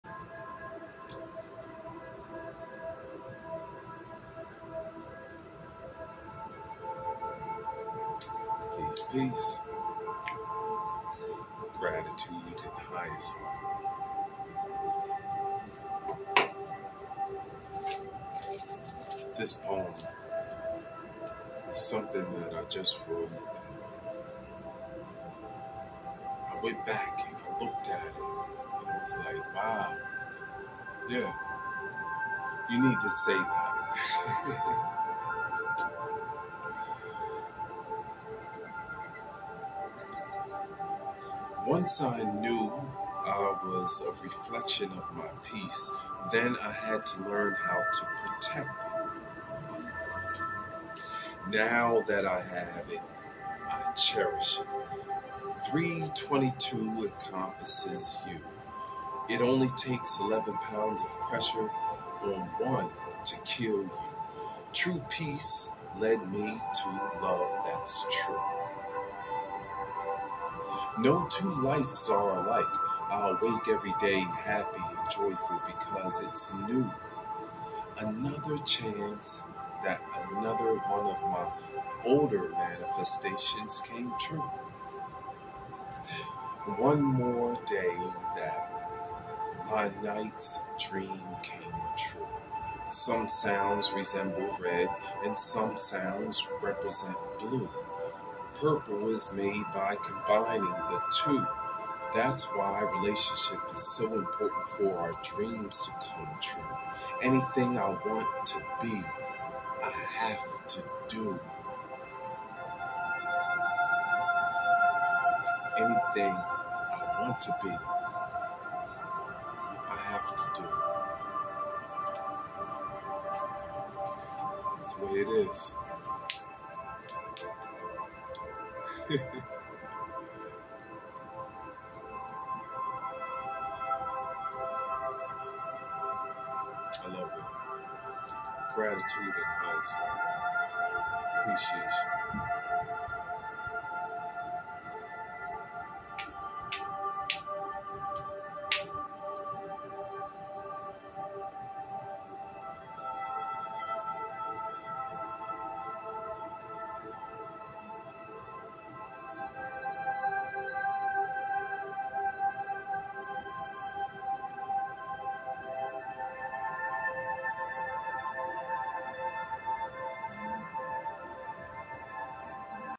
✨lifeisachancetorelatetoself urielriley 💜💎 poetrylover "Life is a chance to relate to self."
💫 Life is a chance to relate to self💫✍🏾🪄🗣️🎵 *Once I knew that I was a reflection of my peace then I had to learn how to protect it. Now That's I have it, I cherish it. 322 encompasses you. it only takes 11lbs of pressure on 1 to kill you. True peace lead me to love that's true. No two(2) lives are alike. I awake everyday happy and joyful because it's new. Another chance that another one of my older manifestations came true. One (1) more day that my nights dream came true. Some sounds resemble red and some sounds represent blue. Purple is made by combining the 2 . That's why relationship is so important for our dreams to come true. Anything I want to be, I have to do.* by Uriel N. Riley 👑🪄✅